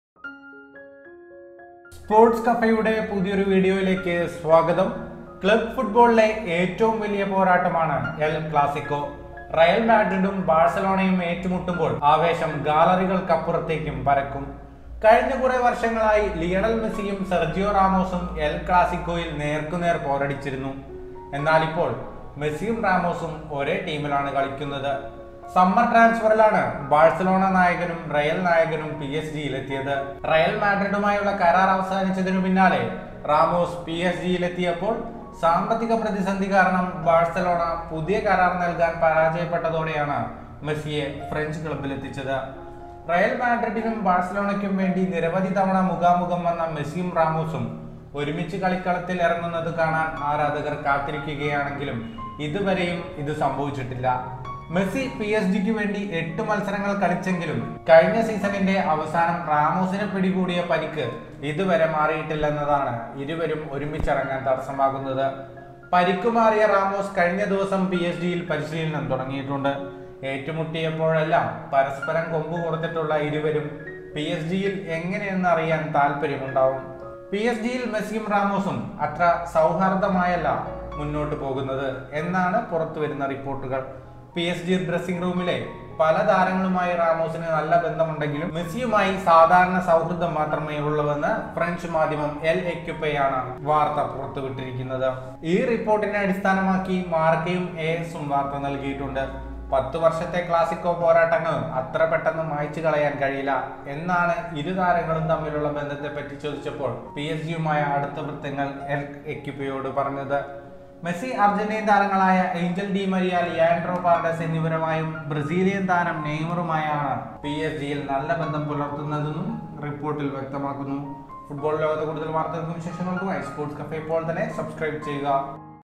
പി എസ് ജിയിൽ എൽ ക്ലാസിക്കോയുടെ അകലംകാത്ത് മെസ്സിയും റാമോസും
Ramos, Messi "didn't erase 10 years of Clásicos from nowhere" - PSG source on pair's relationship
Former Real-Barca rivals, Ramos and Messi both joined PSG this summer following the expiration of their respective contracts and have been getting to grips with life as team mates after years of heated Clásico encounters.

Xavi Hernandez
Ronaldo Malayalam
Gunners Arsenal
Football Malayalam
Chelsea Malayalam
Realmadrid Malayalam